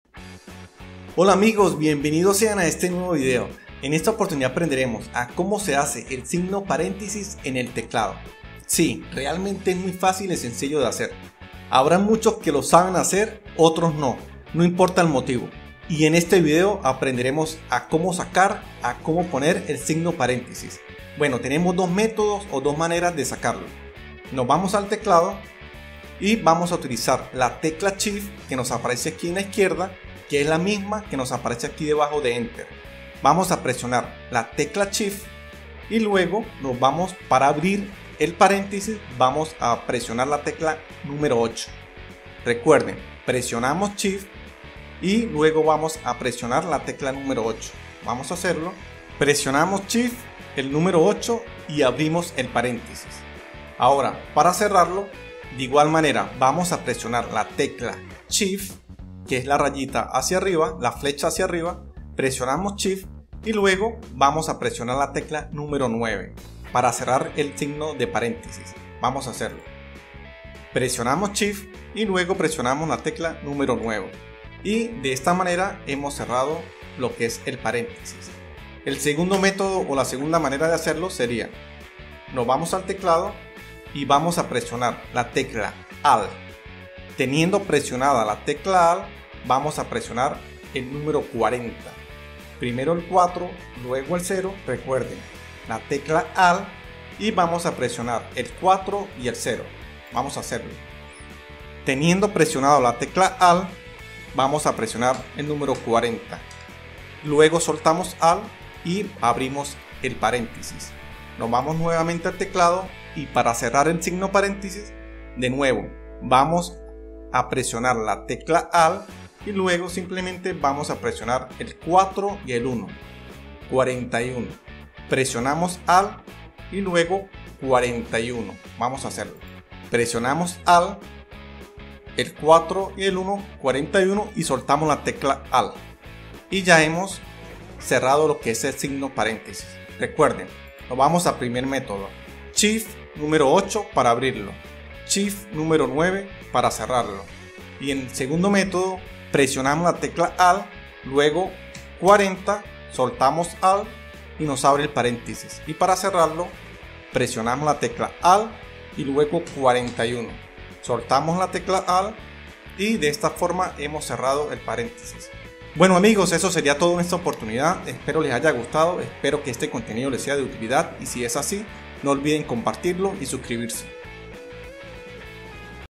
Cómo se Hace el SIGNO PARÉNTESIS en el Teclado
En este vídeo Tutorial Aprenderemos de forma rápida y sencilla, a Cómo se Hace el SIGNO PARÉNTESIS en el Teclado. Como ESCRIBIR un PARENTESIS con el TECLADO